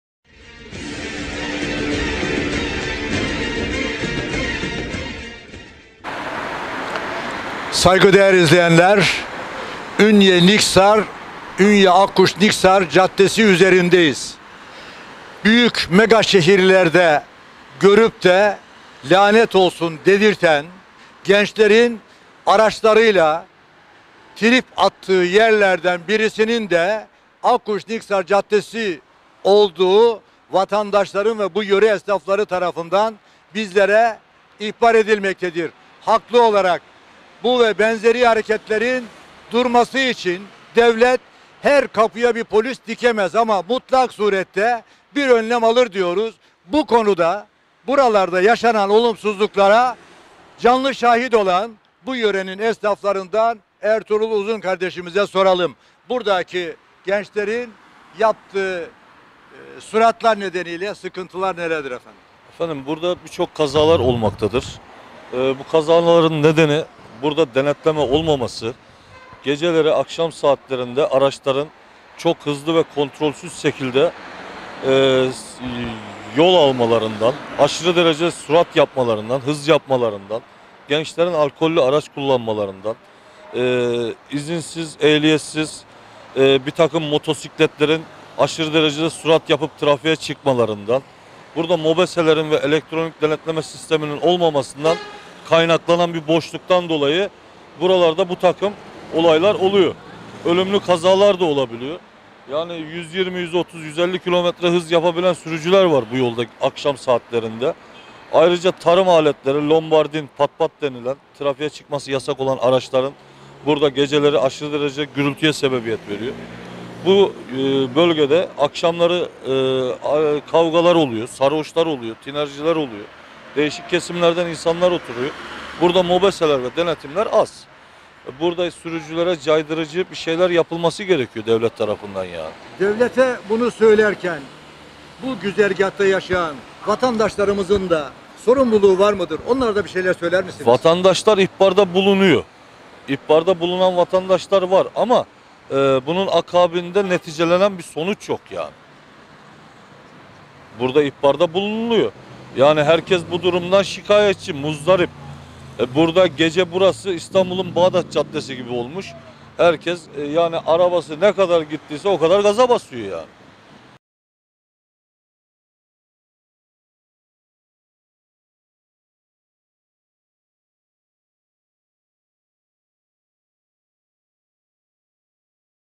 AKKUŞ-NİKSAR CADDESİ SORUMSUZLARIN YARIŞ PİSTİ (Mİ).?
Ünye'nin göbeğinden geçen ve Ünye'nin her çeşit aracının geçtiği en işlek transit araç yolu olan Akkuş Niksar Caddesi hava karardıktan ve özellikle gecenin bir vaktinde kendini bilmez,maceraperest araç sürücülerine hız yapma, yarış yapma,egzozlarını kulakları sağır edercesine aşırı gürültülü biçimde ses çıkarttırma alanıymış gibi kullanılması yöre esnafları ve yöre sakinleri olarak bizleri çileden çıkartıyor diyen Ünyeliler”konuyla ilgili olarak yapılan ihbarlara gerektiği kadar duyarsız kalınması acaba sorusunu akıllarda getirmiyor değil diyerek sitem ediyorlar.